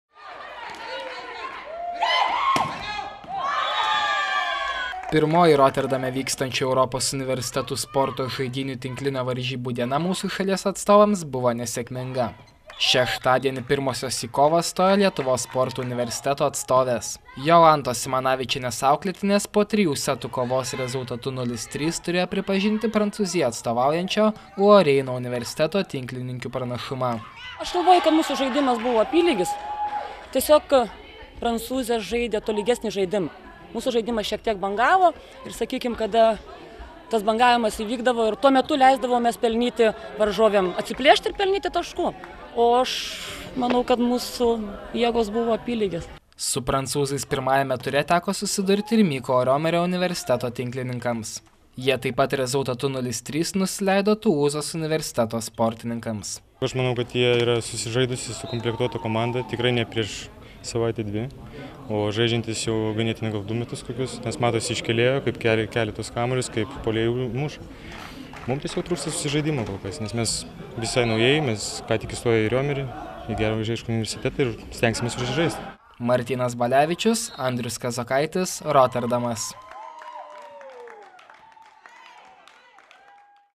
EUSA žaidynių tinklinio turnyro pradžia pažymėta Lietuvos rinktinių nesėkmėmis
Pirmoji Roterdame (Olandija) vykstančių Europos universitetų sporto žaidynių tinklinio varžybų diena mūsų šalies atstovams buvo nesėkminga. Šeštadienį pirmosios į kovą stojo Lietuvos sporto universiteto (LSU) atstovės. Jolantos Simanavičienės auklėtinės po trijų setų kovos rezultatu 0:3 (17:25, 22:25, 17:25) turėjo pripažinti Prancūzijai atstovaujančio Loraino universiteto tinklininkių pranašumą. Su prancūzais pirmajame ture teko susidurti ir Mykolo Romerio universiteto (MRU) tinklininkams. Jie taip pat rezultatu 0:3 (20:25, 15:25, 18:25) nusileido Tulūzos universiteto sportininkams.